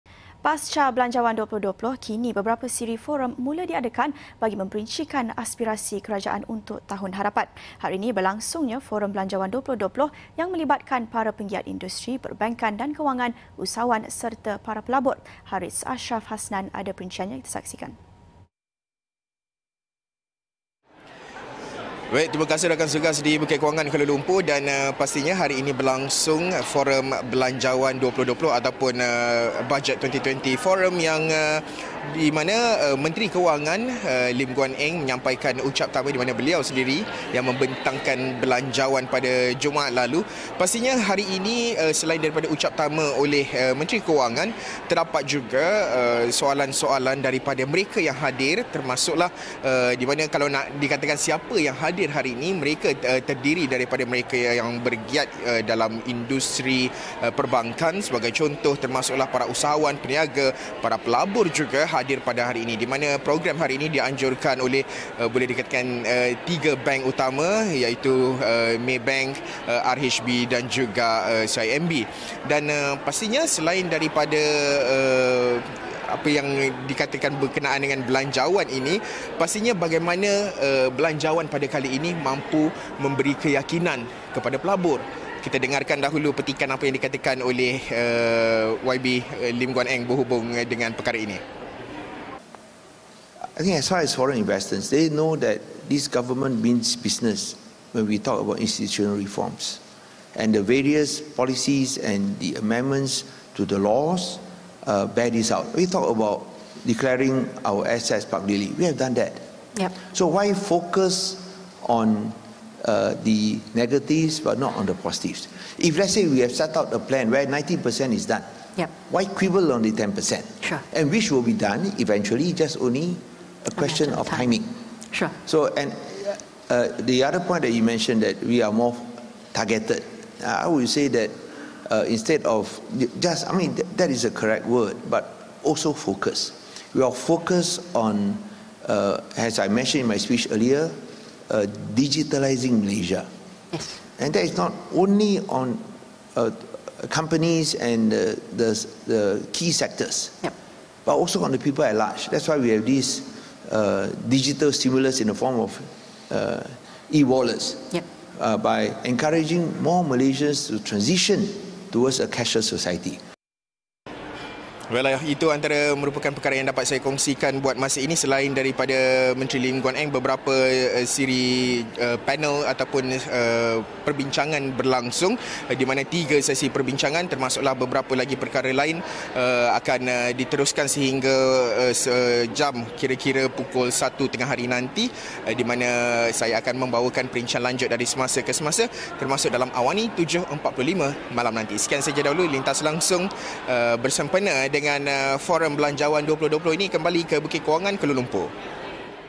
Belanjawan 2020: Keyakinan pelabur asing meningkat
Pasca Belanjawan 2020, kini beberapa siri forum mula diadakan bagi memperincikan aspirasi kerajaan untuk tahun hadapan.

Hari ini berlangsungnya Forum Belanjawan 2020 yang melibatkan para penggiat industri perbankan dan kewangan, usahawan serta para pelabur.

Harits Asyraf Hasnan ada perinciannya.